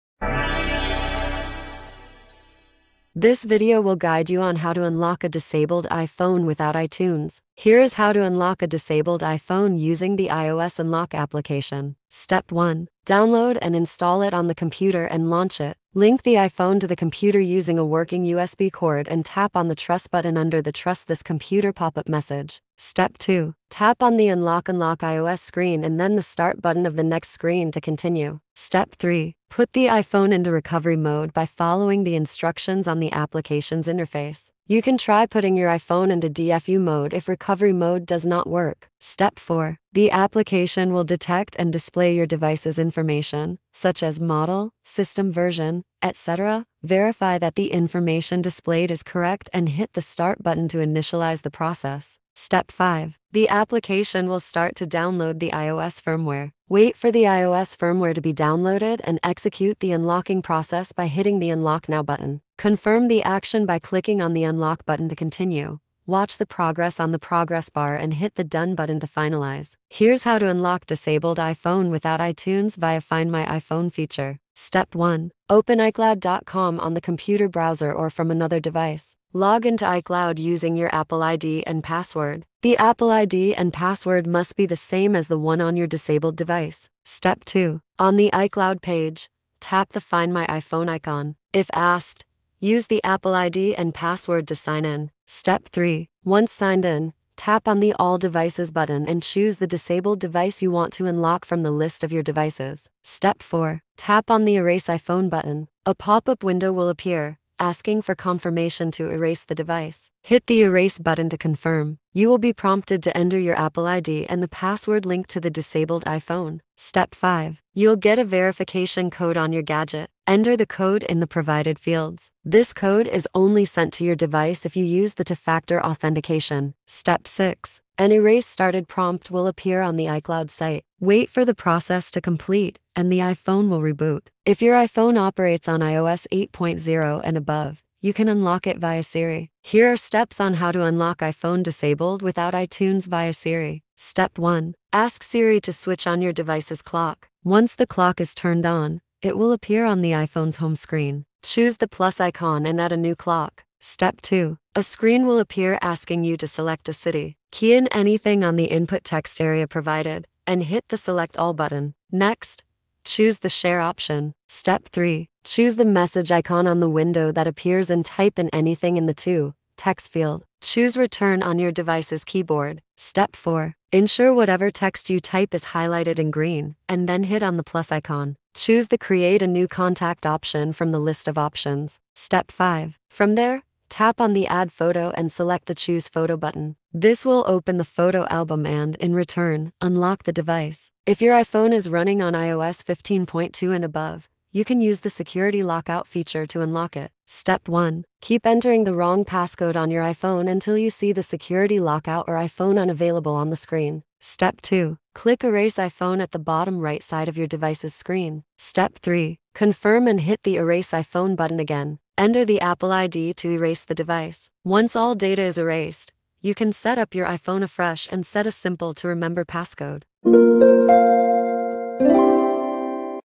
How to Unlock a Disabled iPhone Without iTunes [4 Ways]
Having a locked iPhone can be very frustrating. This video will explain how to unlock a disabled iPhone without iTunes.